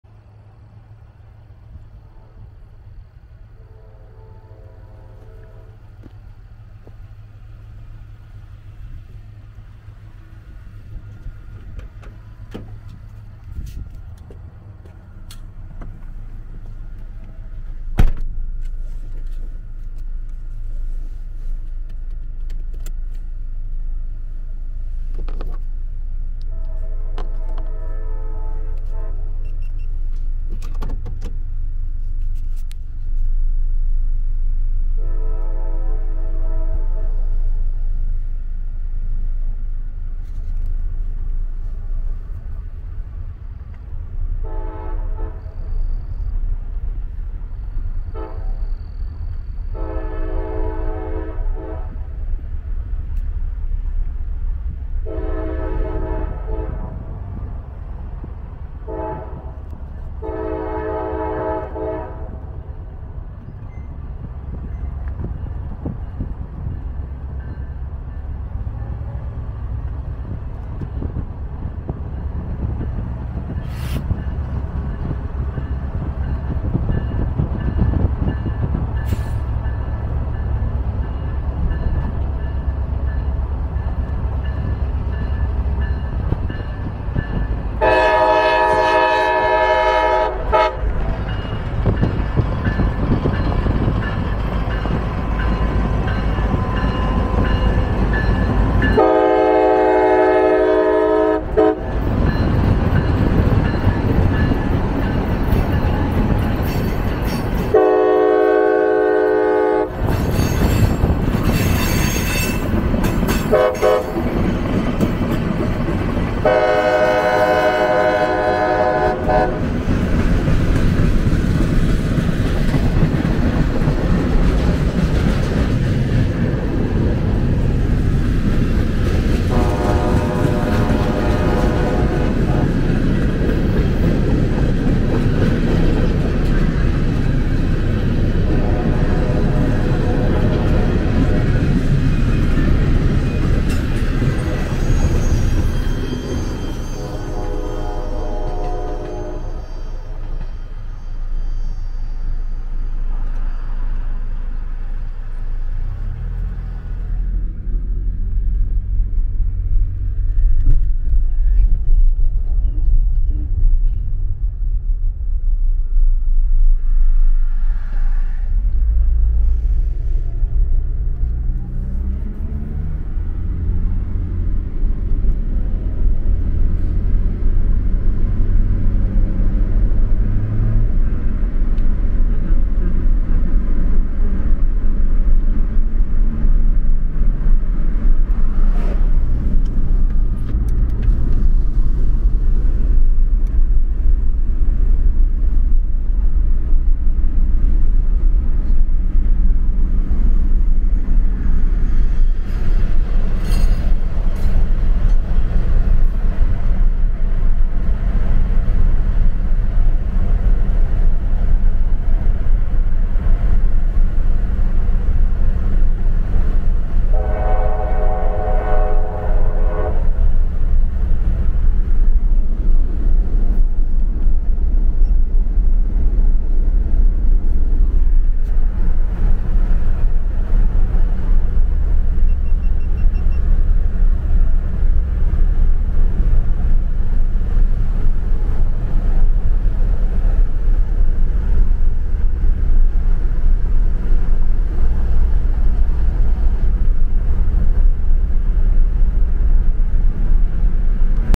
Pacing CSX GP38 No. 2036 Down Forum Lane In Iron Station NC 3-8-21
On the other side of the tracks is Depot Street, where I have paced trains before. Anyway, I thought this was a very nice video.